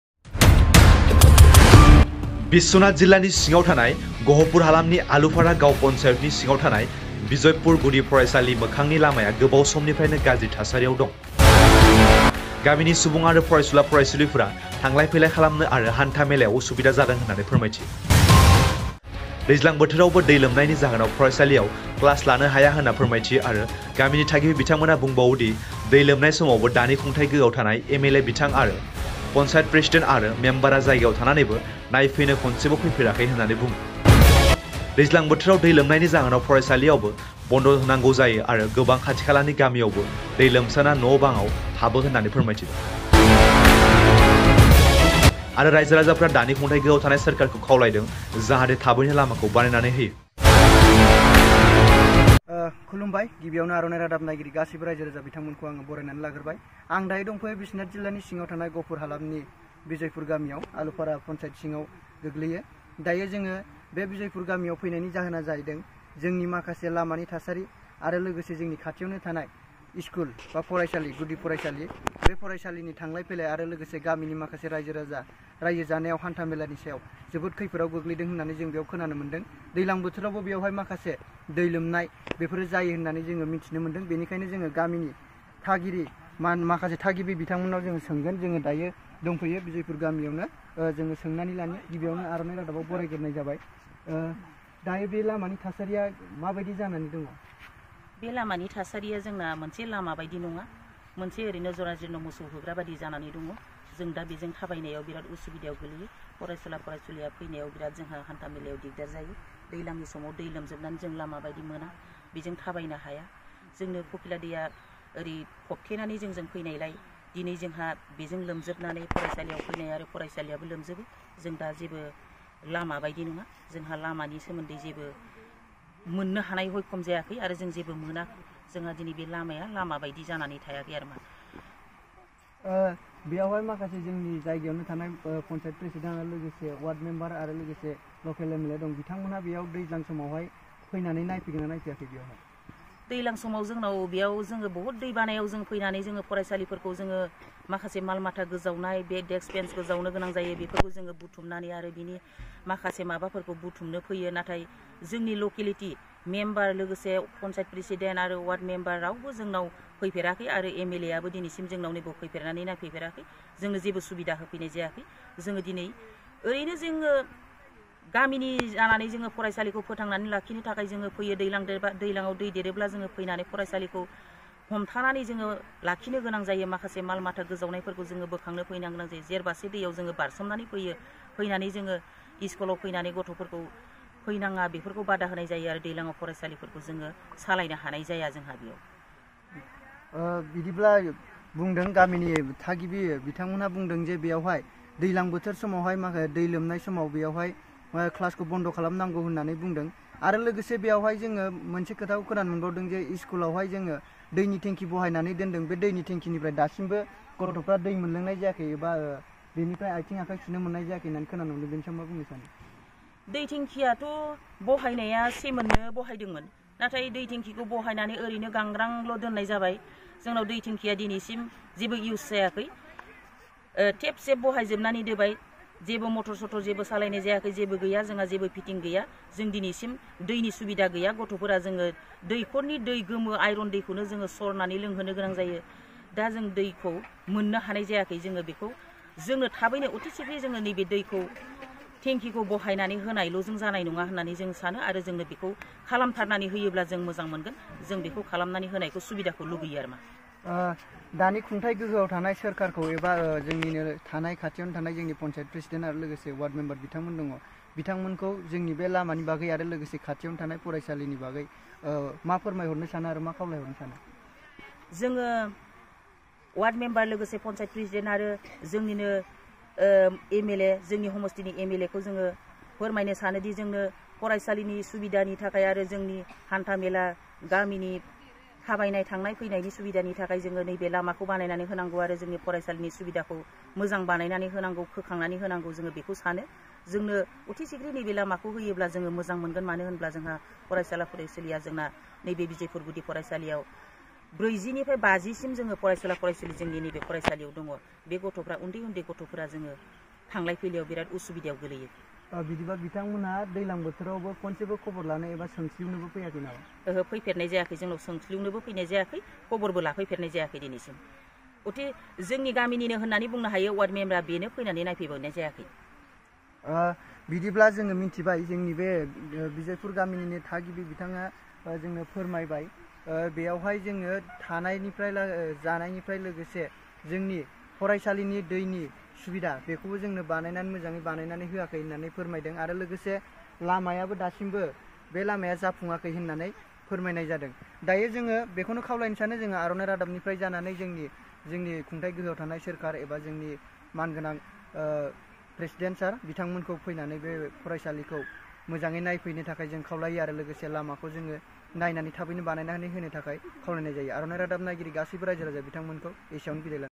बिजयपुर गुदि गुदि फरायसालिनि मोखांआव थानाय लामाया गोबाव समनिफ्रायनो गारजि थासारियाव दं
बिस्वनात जिल्लानि आलुफारा गाव फनसायेडनि सिंङाव थानाय बिजयपुर गुदि गुदि फरायसालिनि मोखांआव थानाय लामाया गोबाव समनिफ्रायनो गारजि थासारियाव दं।